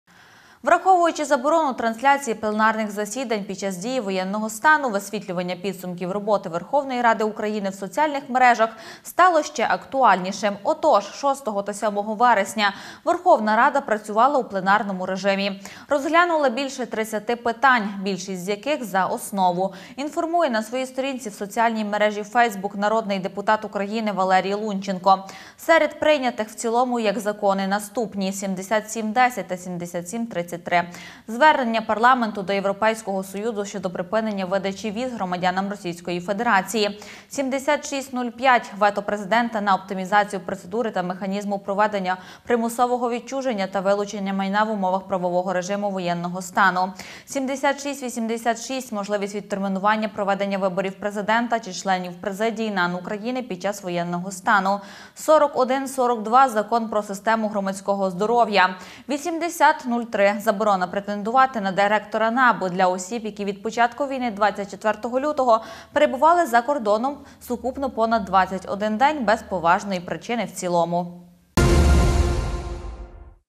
Підсумки засідання
Враховуючи заборону трансляції пленарних засідань під час дії воєнного стану, висвітлення підсумків роботи ВРУ в соціальних мережах стало ще актуальнішим.  Отож, 6 та 7 вересня Верховна Рада працювала у пленарному режимі. Розглянуто більше 30 питань, більшість з яких - за основу – інформує на своїй сторінці у соціальній мережі фейсбук народний депутат України Валерій Лунченко.
Серед прийнятих в цілому як закони - наступні:
7710 та 7733 - звернення парламенту до Європейського Союзу щодо припинення видачі віз громадянам російської федерації (324 та 327).
7605 - вето президента на оптимізацію процедури та механізму проведення примусового відчуження та вилучення майна в умовах правового режиму воєнного стану (262).
7686 - можливість відтермінування проведення виборів президента та членів президії НАН України під час воєнного стану // в цілому (325).
4142 - закон про систему громадського здоров’я // в цілому (228).
 8003 - заборона претендувати на директора НАБУ для осіб, які після початку війни (24 лютого) перебували за кордоном сукупно понад 21 день без поважної причини // в цілому (285).